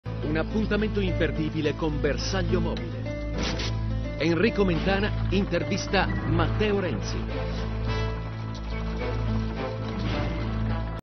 Enrico Mentana intervista Matteo Renzi - Questa sera alle 22.40
Questa sera alle 22.40 il direttore Enrico Mentana intervista il Premier Matteo Renzi